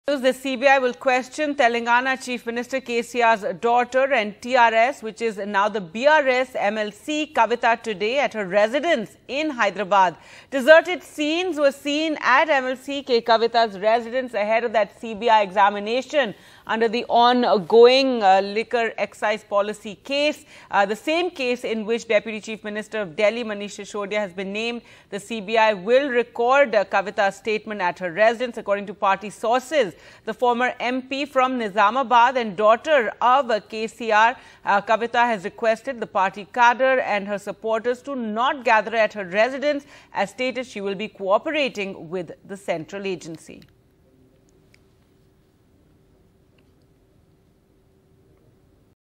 KCR's Daughter To Be Questioned By CBI Today In Delhi Liquor Policy Case
Security was heightened near the residence of Telangana Rashtra Samithi MLC K Kavitha hours ahead of her questioning by the Central Bureau of Investigation in connection with the Delhi Excise Policy scam case on Sunday.